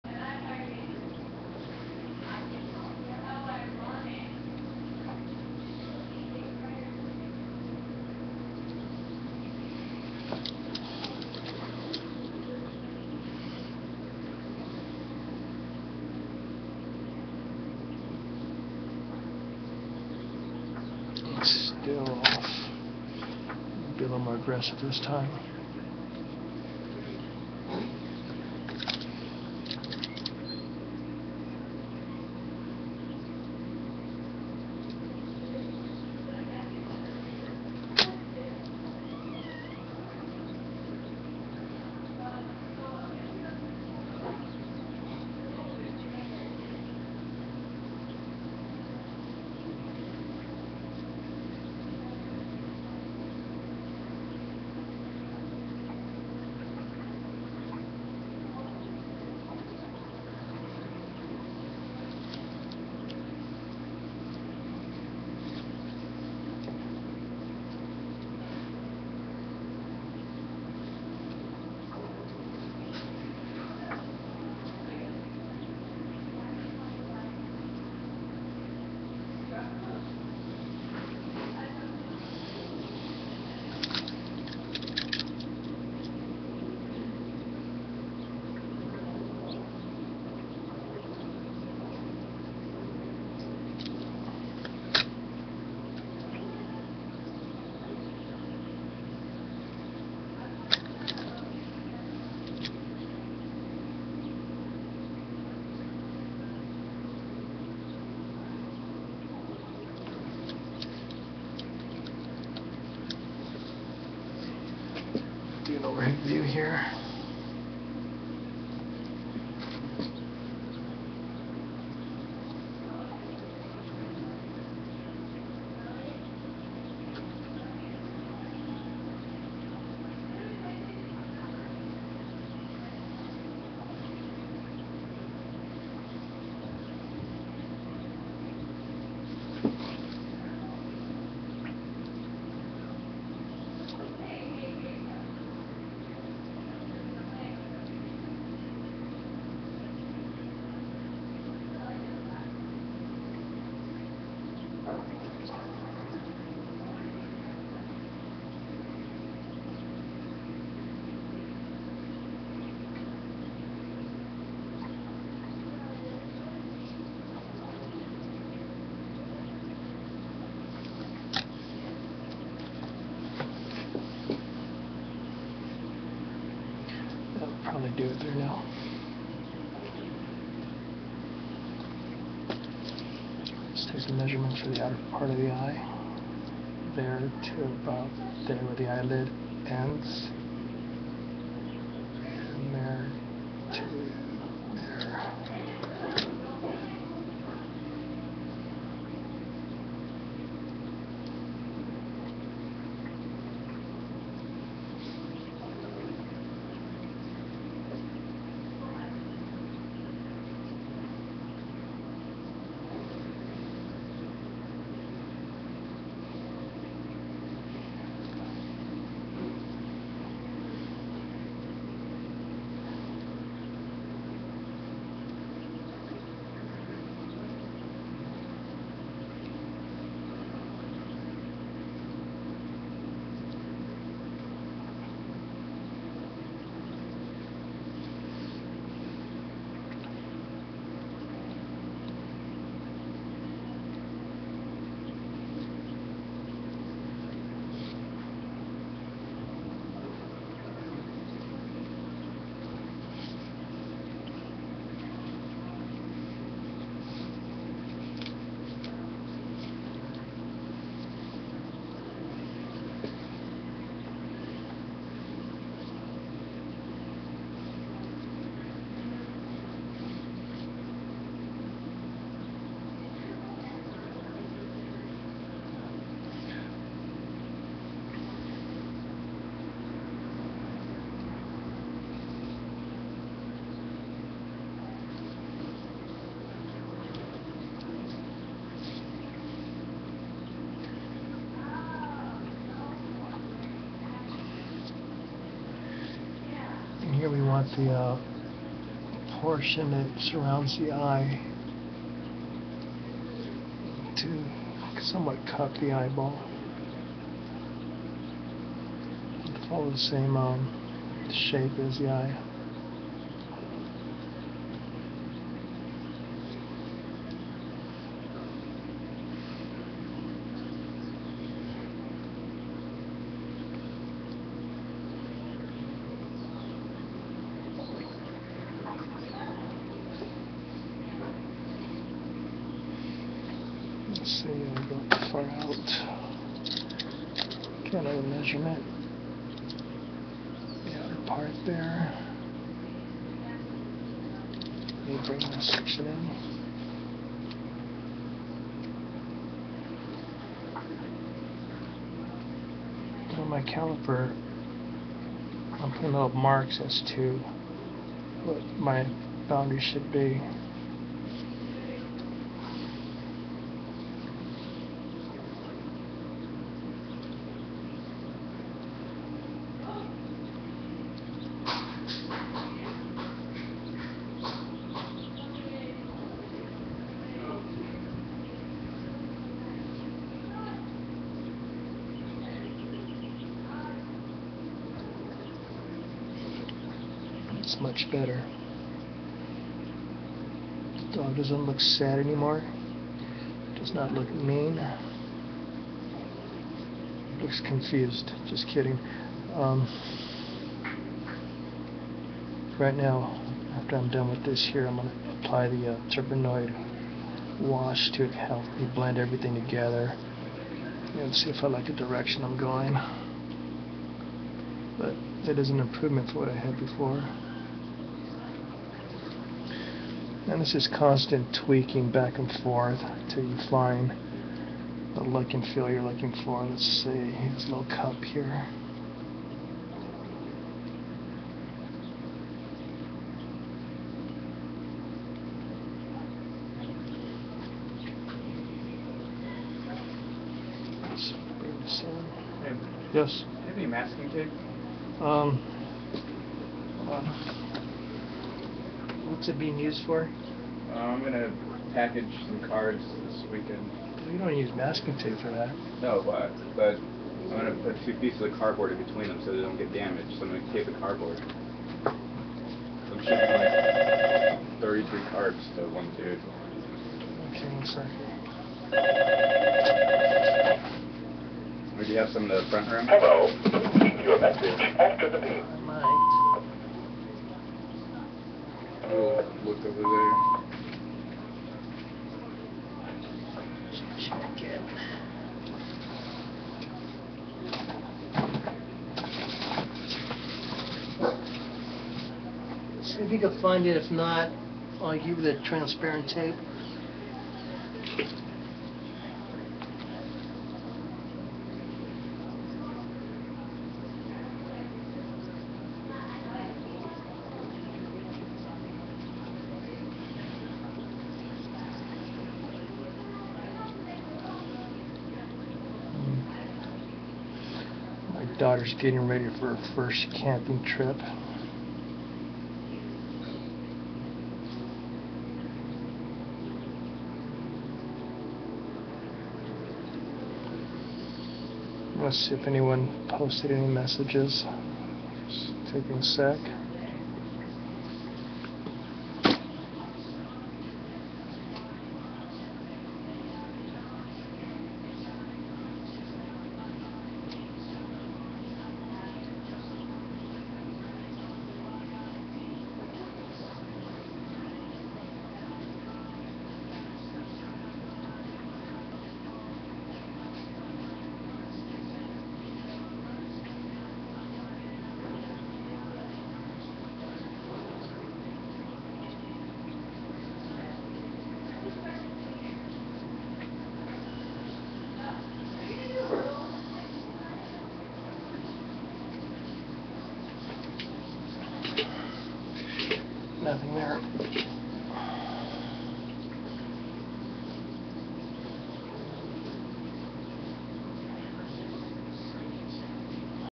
no 045 Ridgeback head sculpting
vistit my blog to view more of my art - Tweaking the head section. Correcting the look and feel of the eyes and ... (More) Tweaking the head section. Correcting the look and feel of the eyes and over all head look.